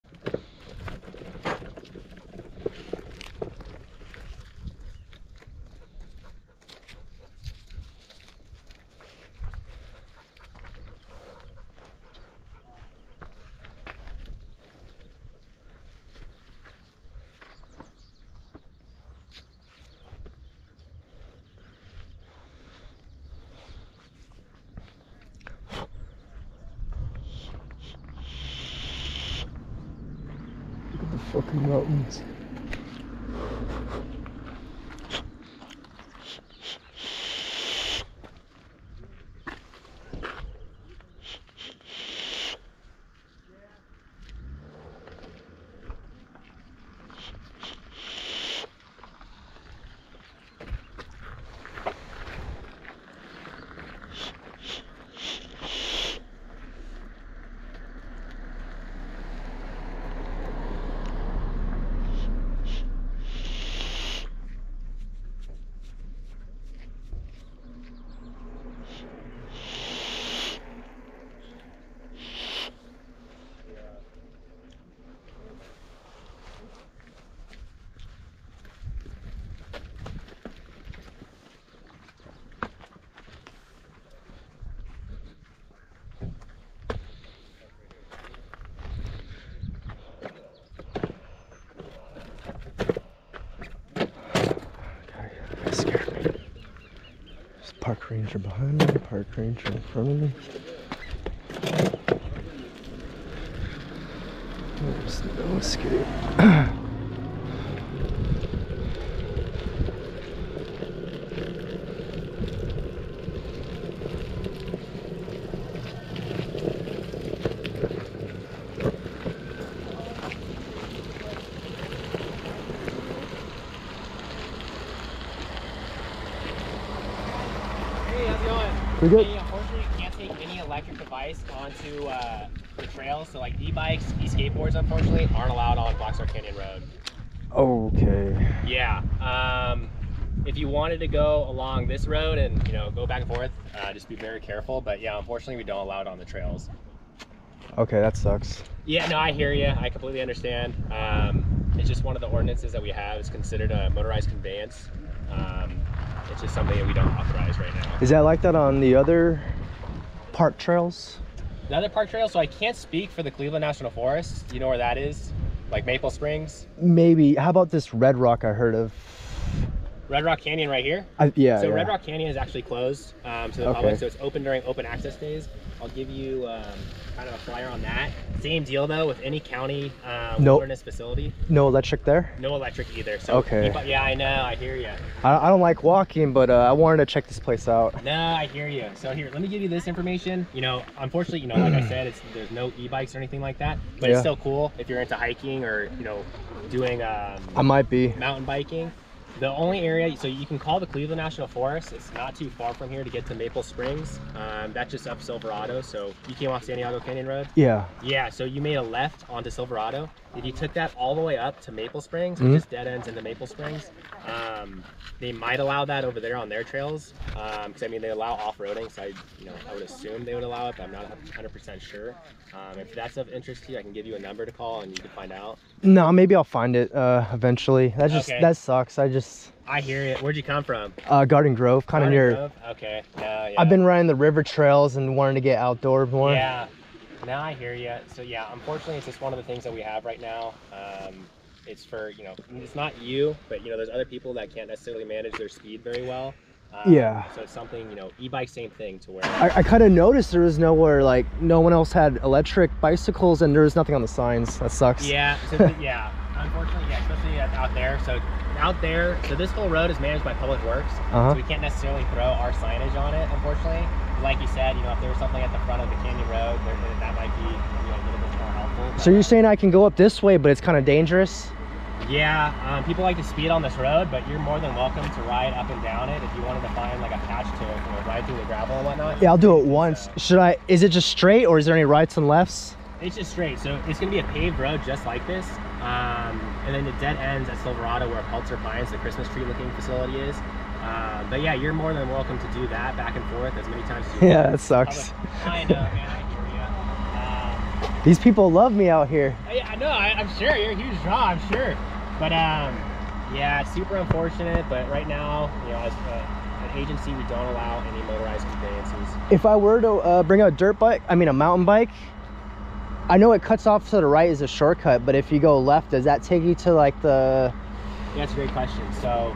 No Electric Vehicles(esk8) in Black Star Canyon Trail
i'm only vaping so much because the Park Ranger behind me caught me off guard and I wanted to keep acting normal.